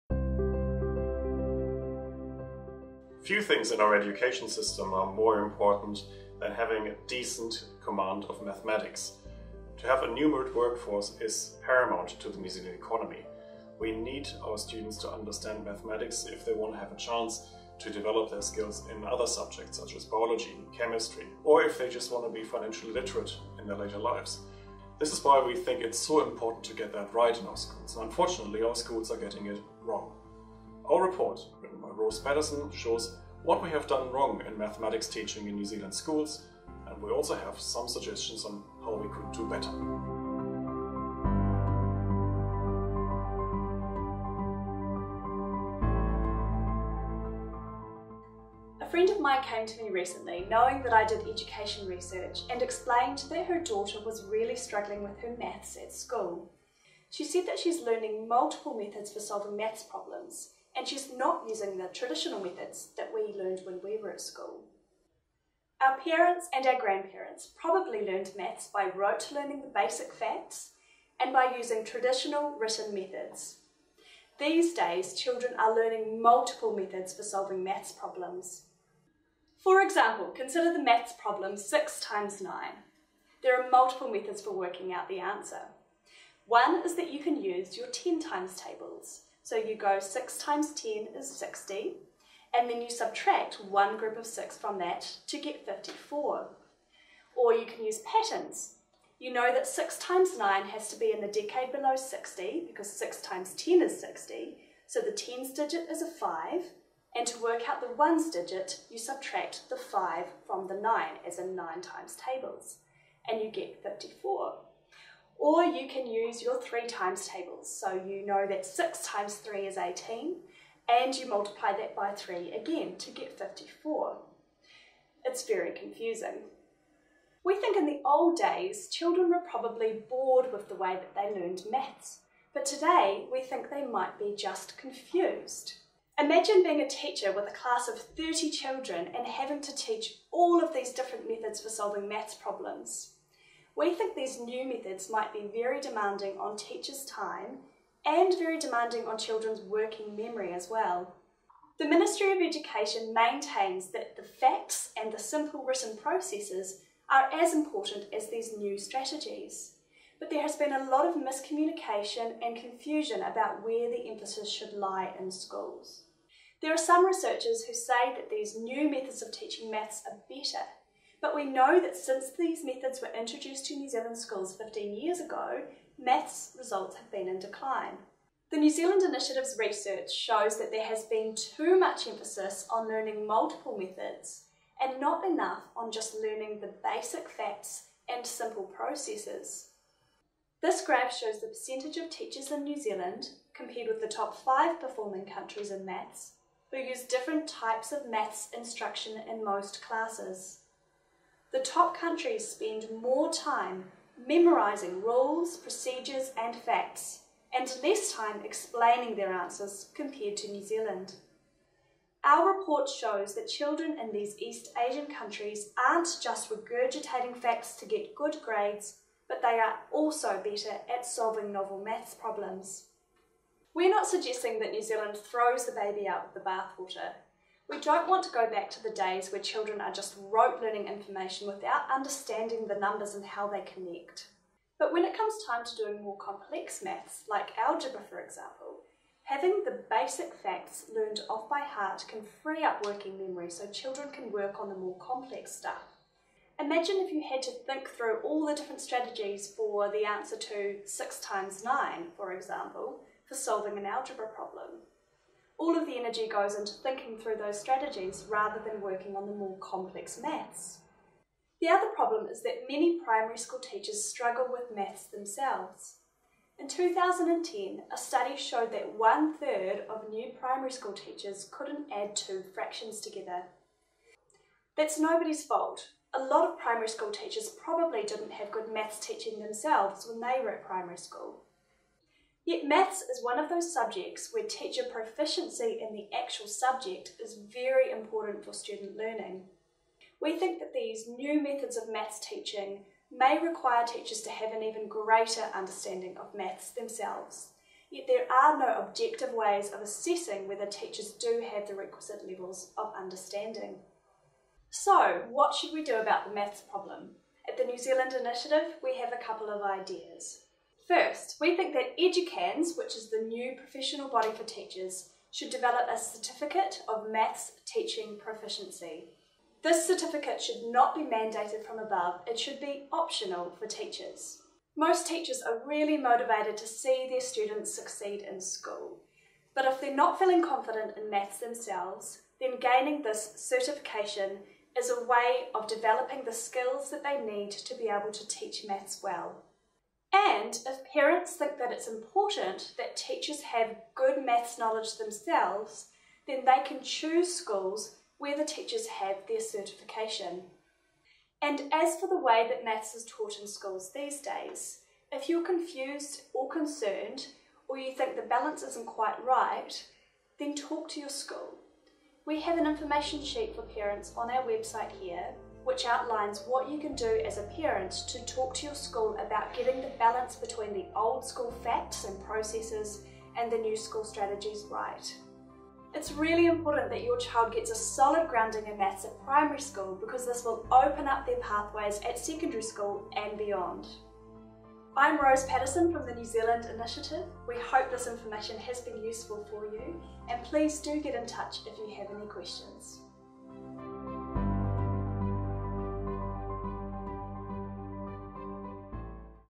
Un(ac)countable: Why millions on maths returned little
Author Rose Patterson describes the findings of her new report, Un(ac)countable: Why millions on maths returned little. The report covers the history of the government's Numeracy Project implemented in most primary schools 15 years ago, at a cost of $70m. The programme teaches multiple ways of solving maths problems.

The results of the Numeracy Project? Children's basic mathematic skills are in decline.